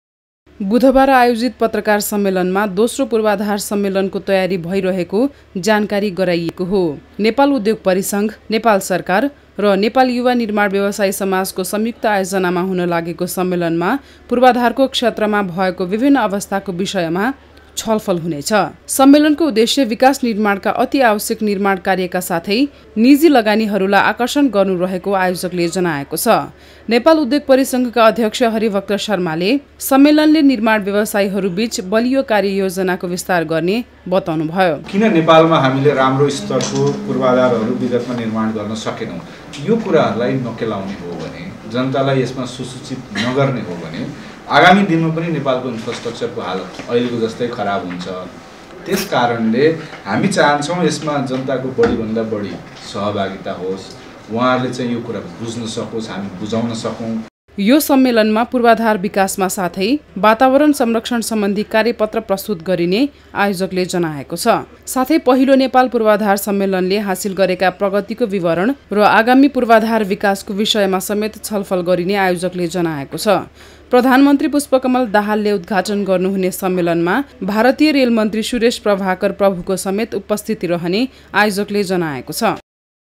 दोस्रो नेपाल पुर्वाधार सम्मेलन काठमाडौमा हुने-NEWS 24
दोस्रो नेपाल पुर्वाधार सम्मेलन आगामी फागुन ८ र ९ गते  राजधानीमा हुने भएको छ । नेपाल उद्योग परिसंघ,नेपाल सरकार र नेपाली युवा निर्माण ब्यवसायी समाजको संयुक्त आयोजनामा पुर्वाधार सम्मेलन हुन लागेको हो । बुधवार आयोजित पत्रकार सम्मेलनमा दोस्रो पुर्वाधार सम्मेलनको तयारी भइरहेको जानकारी गराइएको हो । नेपाल उद्योग परिसंघ, नेपाल सरकार र नेपाली युवा निर्माण ब्यवसायी समाजको संयुक्त आयोजनामा हुन लागेको सम्मेलनमा पुर्वाधारको क्षेत्रमा भएको बिभिन्न अवस्थाको बिषयमा छलफल हुनेछ । सम्मेलनको उदेश्य विकास निर्माणका अति आवश्यक निमाणर््ा कार्यका साथै नीजि लगानीहरुलाई आर्कषण गर्नु रहेको आयोजकले  जनाएको छ । नेपाल उद्योग परिसंघका अध्यक्ष हरिभक्त शर्माले सम्मेलनले निर्माण व्यवसायीहरुबीच बलियो कार्ययोजनाको विस्तार गर्ने बताउनुभयो ।